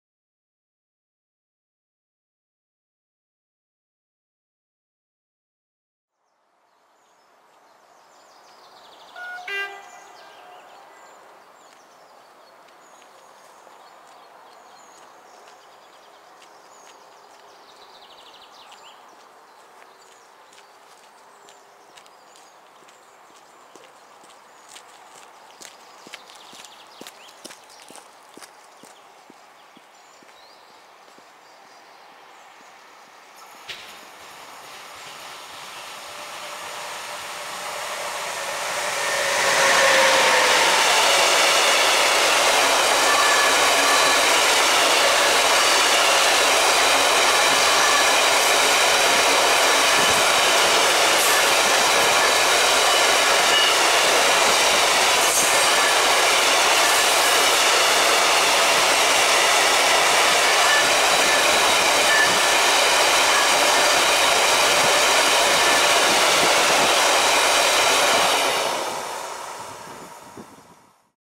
Dragon2 E6ACTa-007 z węglarkami w kierunku Poznania. +RP1
Newag Dragon2 E6ACTa-007 z węglarkami mija na biegu most nad rzeką Sąsiecznicą w Żmigrodzie w kierunku Poznania.

Województwo: Dolnośląskie
Miejscowość: Żmigród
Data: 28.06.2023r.
Godzina: 05:45
Przewoźnik: LTC
D29: 271 (Wrocław - Poznań)
Montaż: Camtasia Studio